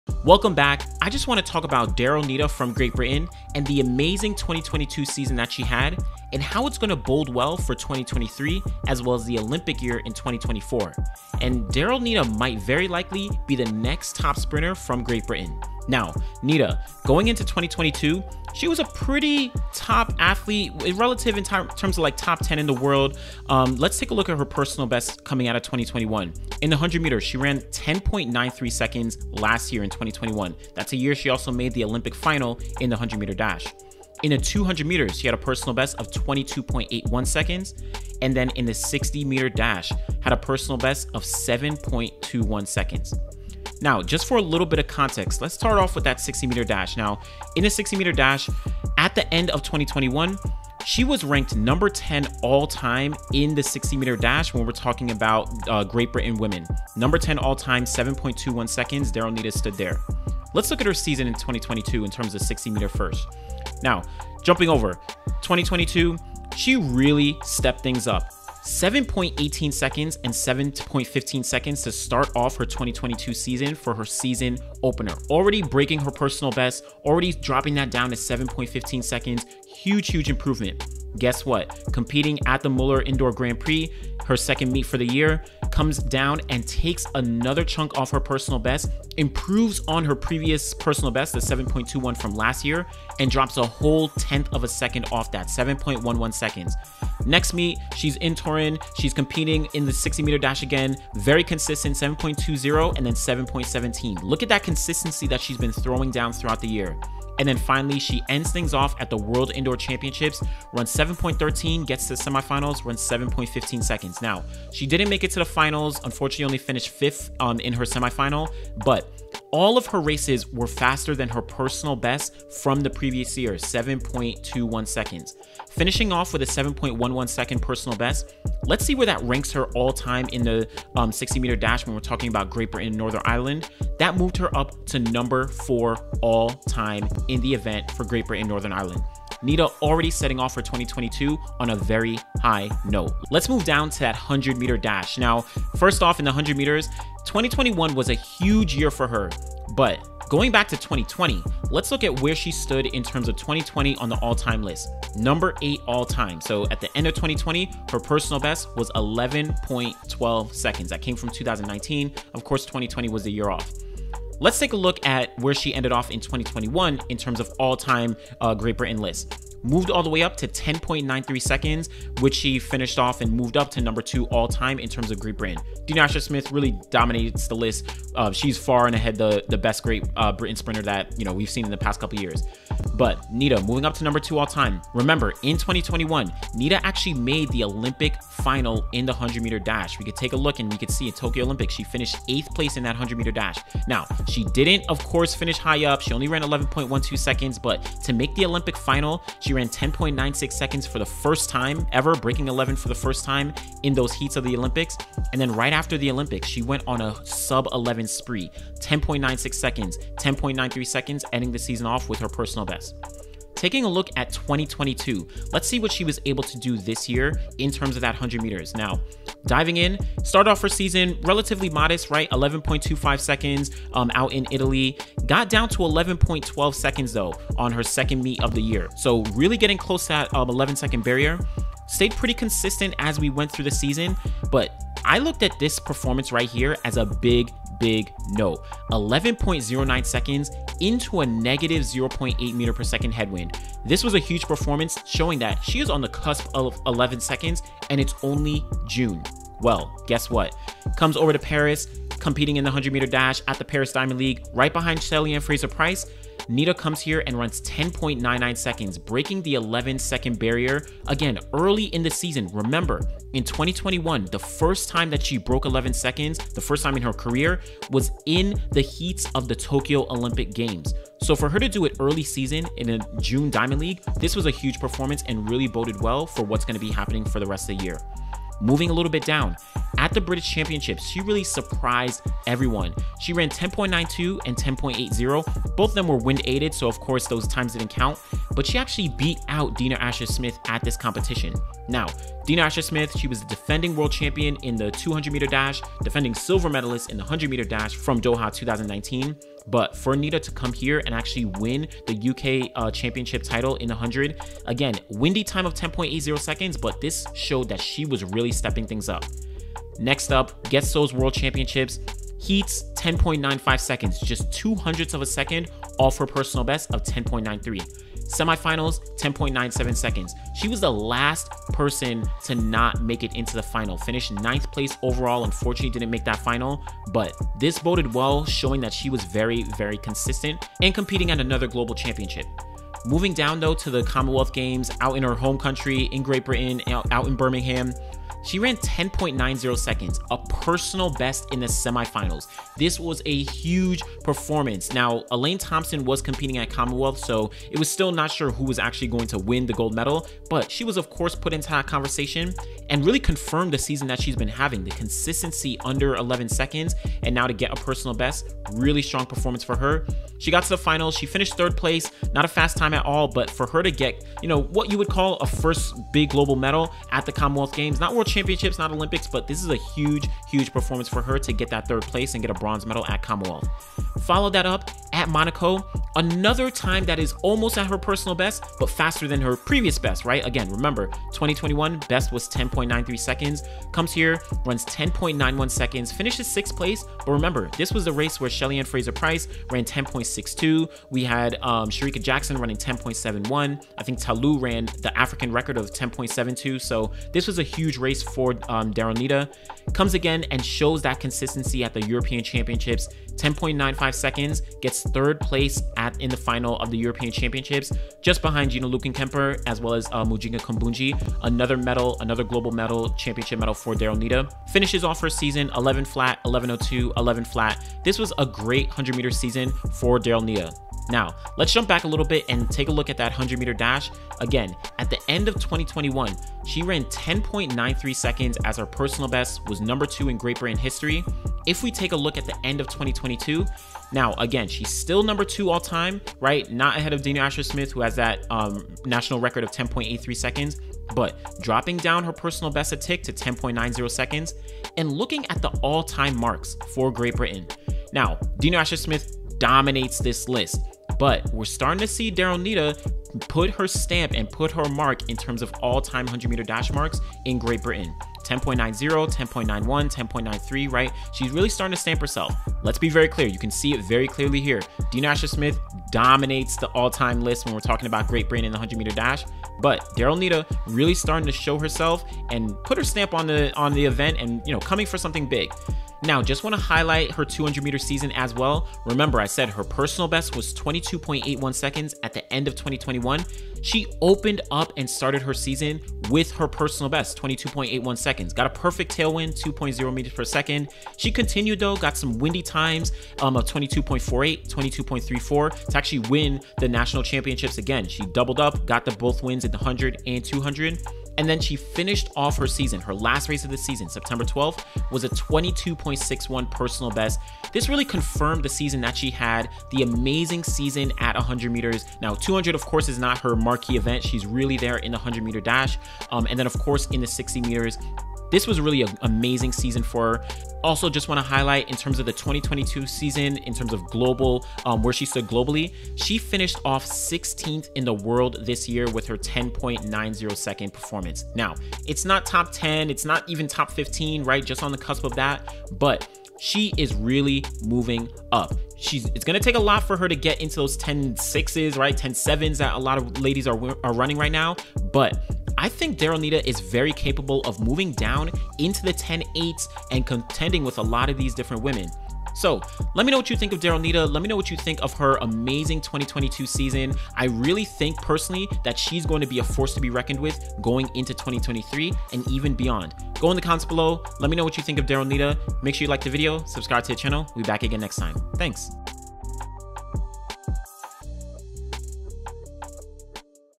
Will Daryll Neita’s become Great Britain's Next Top 100m Sprinter? | 2023 Athletes to Watch
Building off her 2021 season where she made it to the Olympic final, Daryll Neita of Great Britain had an Amazing 2022 season where she improved her personal best in the 60m, 100m and 200m and won Bronze medals at the Commonwealth Games and European Championships.

Will Neita be one of the top sprinters in the world in 2023?